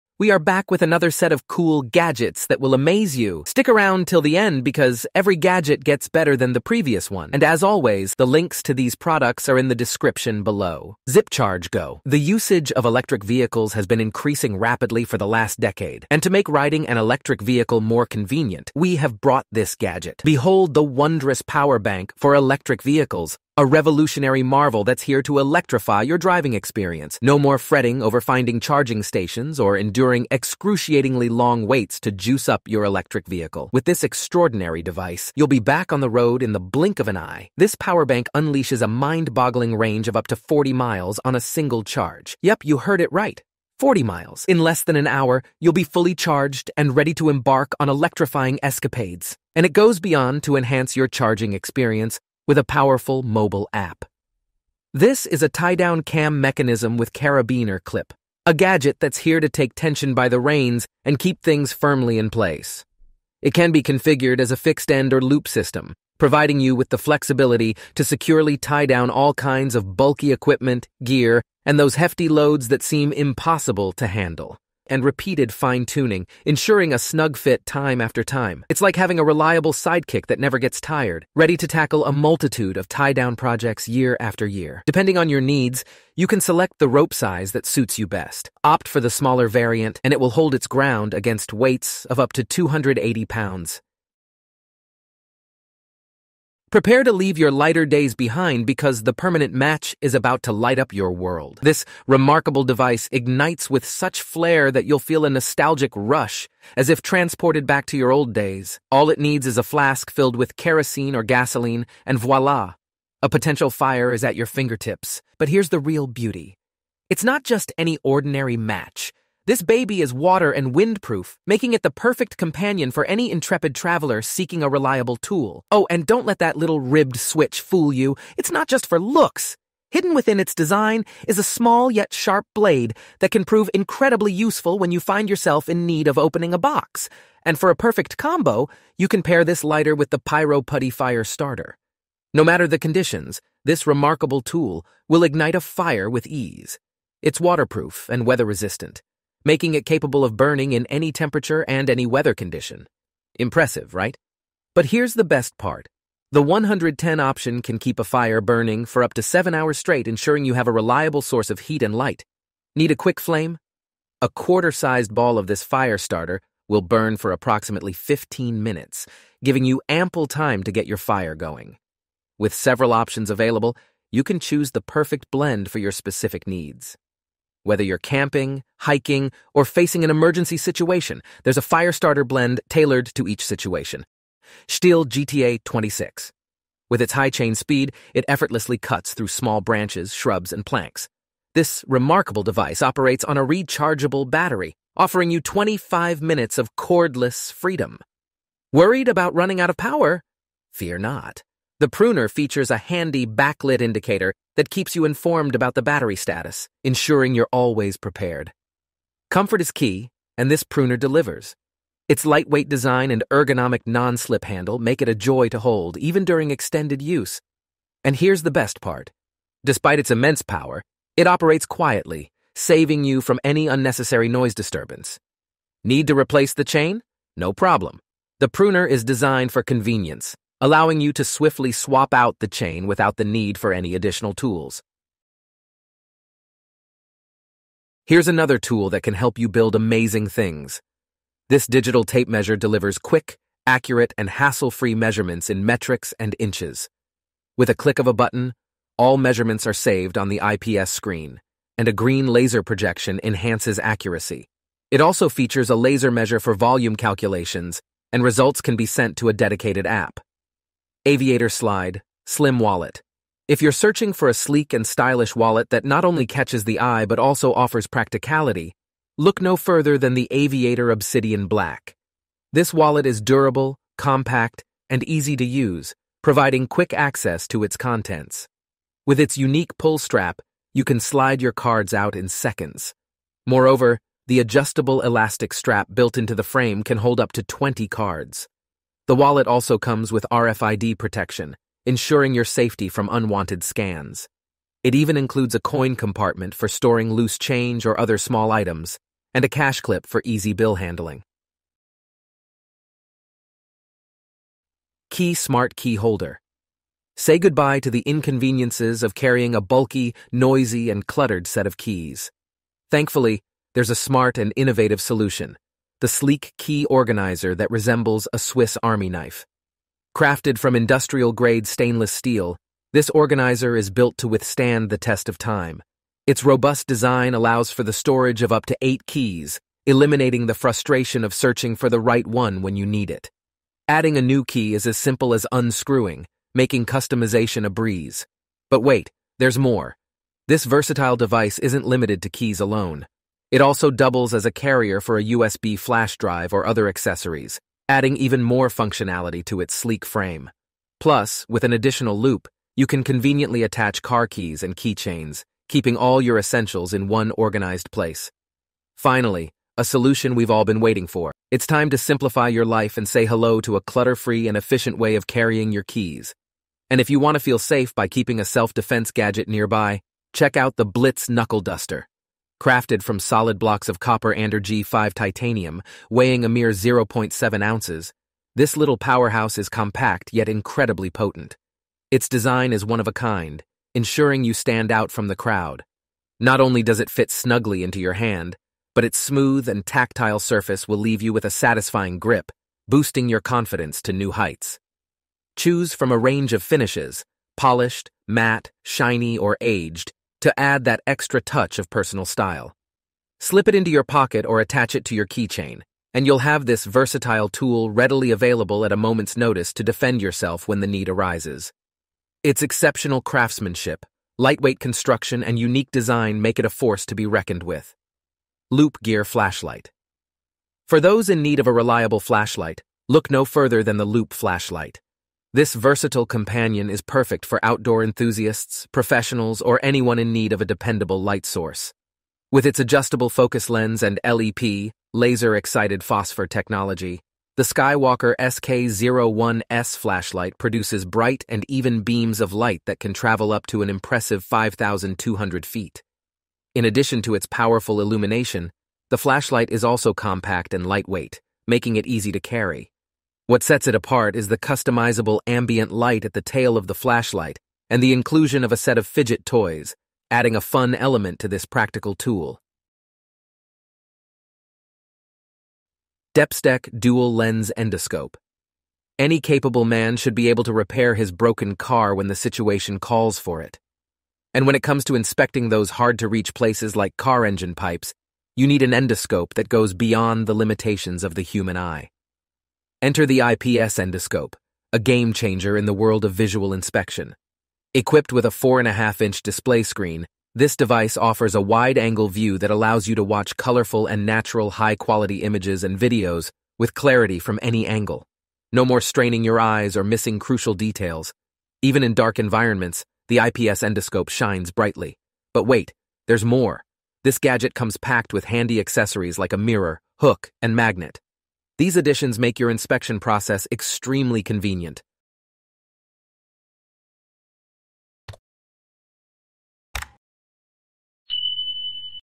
17 COOLEST Gadgets For High Value Men
Gadget's for men discussed on this video:

Zip Charge Go: N/A
Tie Down Clip
Permanent Match
Pyro Putty
Stihl GTA 26
Acegmet DTx 10
Aviator Slide Slim Wallet
Key Smart Key Holder
BLITZ Knuckle Duster
Loop Gear Skywalker SK01s Flashlight
Depstech Dual Lense Endoscope